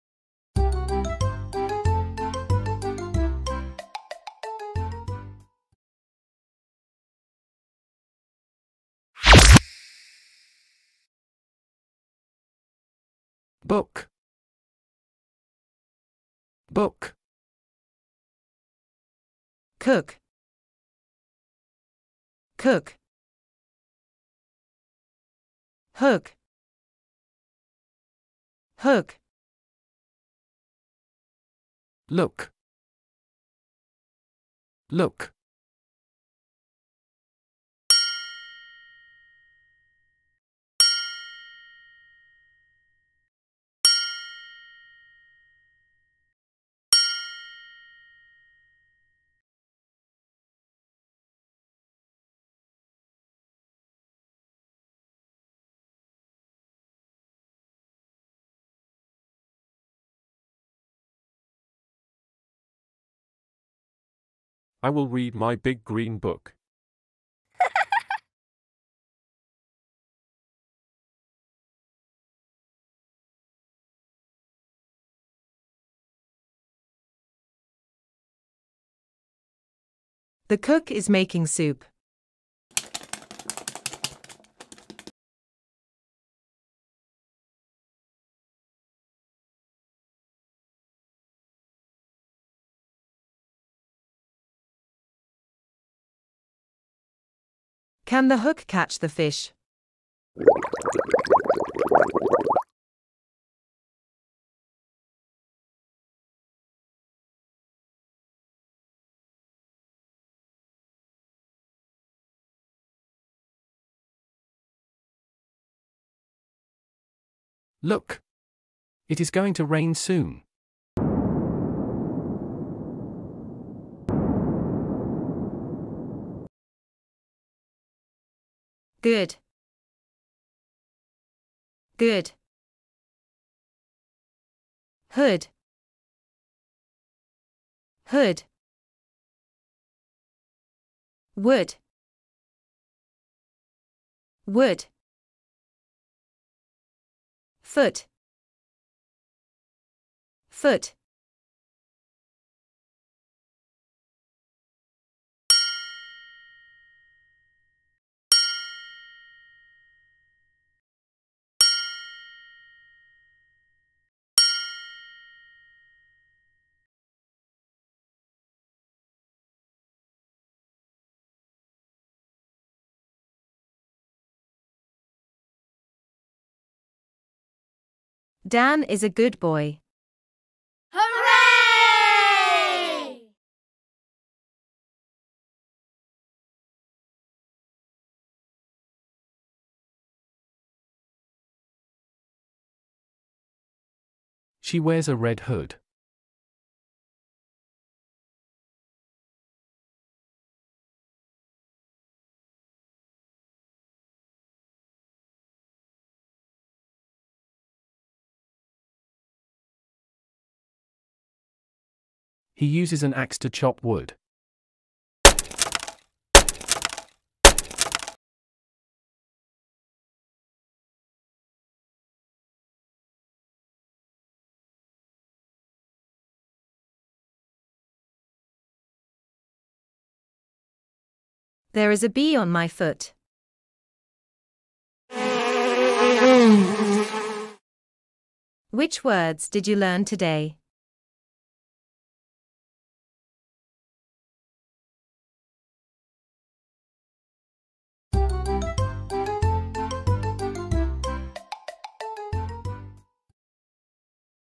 oo Short Vowel Digraph
Teaching vowel digraphs can be challenging, as these combinations of vowels often create unique sounds. However, with the right strategies and resources, educators can help students master these essential language skills.

for strategies on how to teach Digraphs.